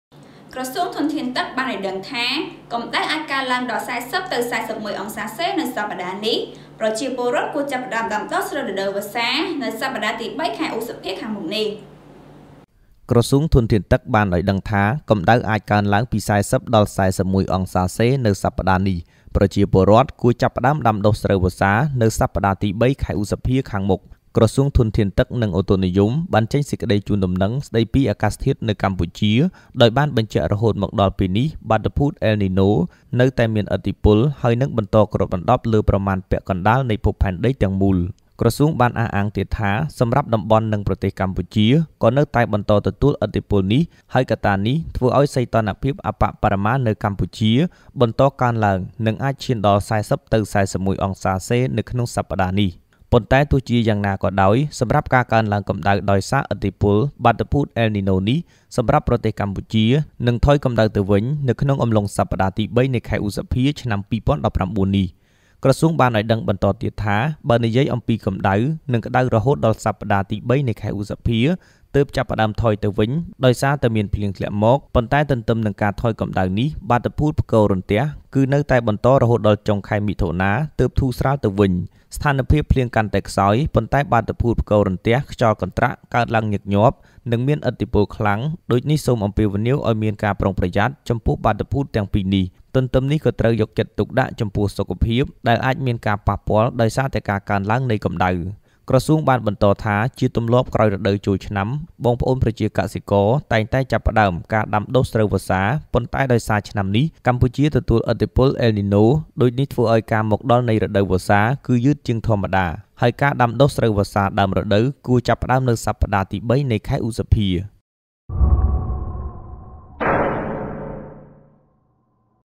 ក្រសួងធនធានទឹក៖ កម្តៅអាចកើនឡើងដល់៤០-៤១អង្សាសេ នៅសប្តាហ៍នេះ, 24 April , 2019
ក្រសួងធនធានទឹក៖ កម្តៅអាចកើនឡើងដល់៤០-៤១អង្សាសេ នៅសប្តាហ៍នេះ, ប្រជាពលរដ្ឋគួរចាប់ផ្តើមដាំដុះស្រូវរដូវវស្សានៅសប្តាហ៍ទី៣ ខែឧសភាខាងមុខ!
អាន៖ លោក រិទ្ធ អារុណ្ណាទី , កញ្ញា ស ដាឡែន,កញ្ញា វ៉ាត ស្រីមួយ ,លោក យ៉ាន យាត
បច្ចេកទេស៖ ទូច សុវី, ធូ សុខឡេង, ឡេង ធីតា
គ្រប់គ្រងព័ត៌មាន និងបច្ចេកទេស៖ វ៉ាន់ វីរៈ
គ្រប់គ្រងការផលិត៖ លោក ស ស៊ីណា
រក្សាសិទ្ធដោយ មន្ទីរព័ត៌មានខេត្តត្បូងឃ្មុំ